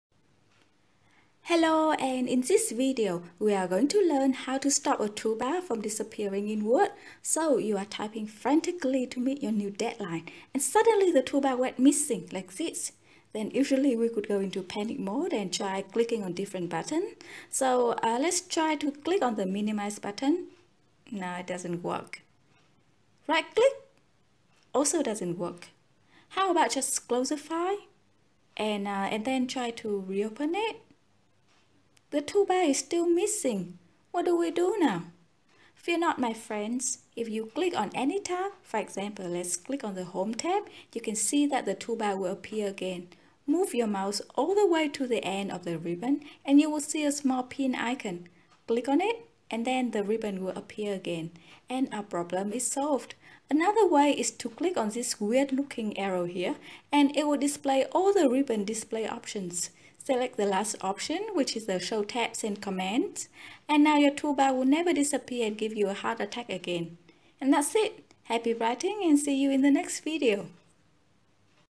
How to stop the toolbar menu from disappearing in Word, Excel or PowerPoint?
So, you are typing frantically to meet your deadline, and suddenly the toolbar went missing. Tricky situation, right?  But fear not my friends.  In this video, I will show you 2 easy ways to restore a missing toolbar menu permanently, so it will never give you any sudden heart attack again.  Spend just 1 minute to find out how.

Minh Nguyen (Sky Aries)